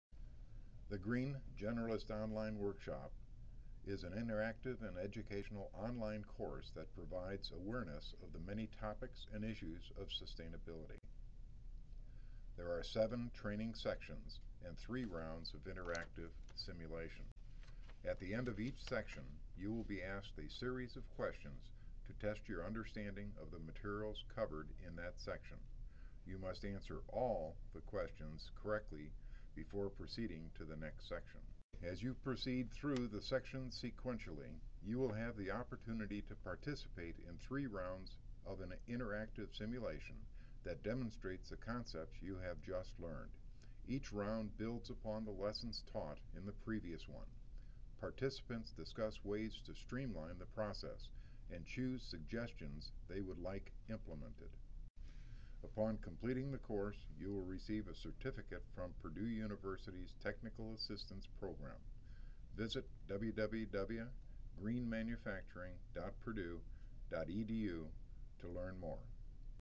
Green training and certification from Purdue University Green Enterprise Development
A Brief Overview of the TAP Green Generalist training offered by Purdue University Technical Assistant Program's Green Enterprise Development. Offering onsite, online, public workshops, and train the trainer methods of training the workforce in green concepts. Created April 14, 2011.